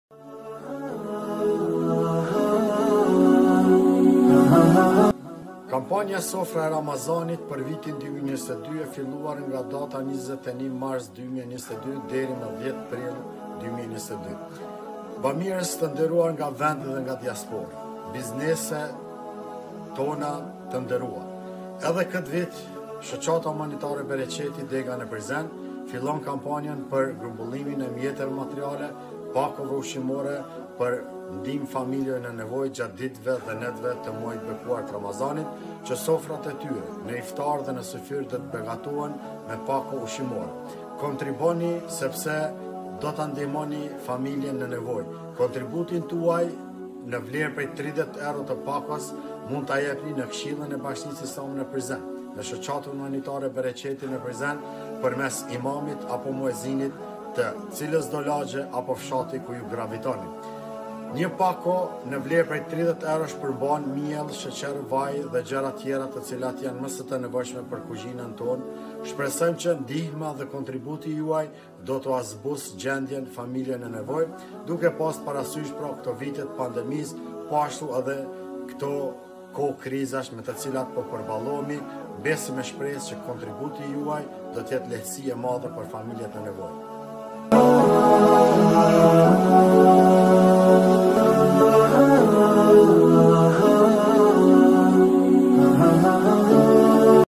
Sofra e Ramazanit 2022 - Bereqeti në Prizren
Bëhu pjesë e kampanjës Sofra e Ramazanit 2022 duke kontribuar me 30 euro për një pako.
Kontributin tuaj mund ta jepni në Shoqatën Bereqeti dega në Prizen.